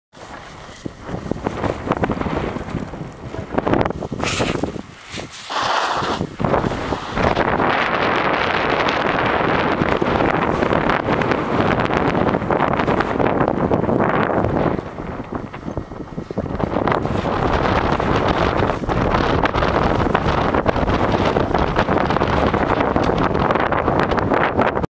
Wind, snow, and roll
A hike up the hill followed by a roll down.